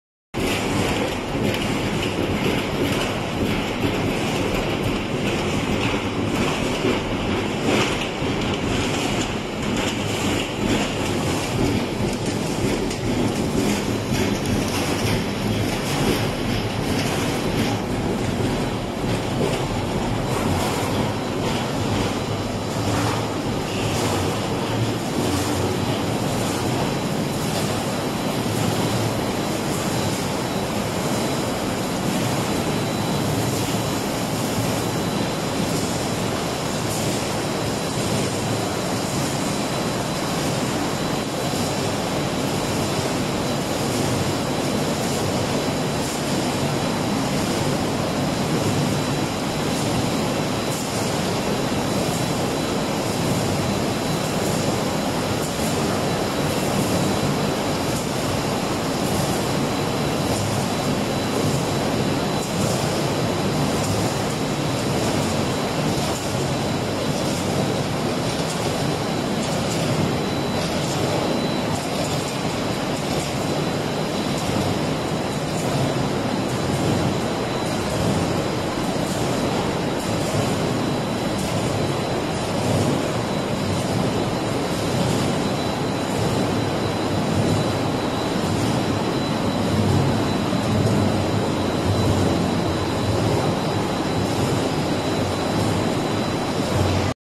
Mango juice processing making line
The line is for making the mango juice.

84798210   Mixing machine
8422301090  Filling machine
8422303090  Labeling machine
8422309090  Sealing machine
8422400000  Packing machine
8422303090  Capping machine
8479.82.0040 food Mixing, kneading or stirring machine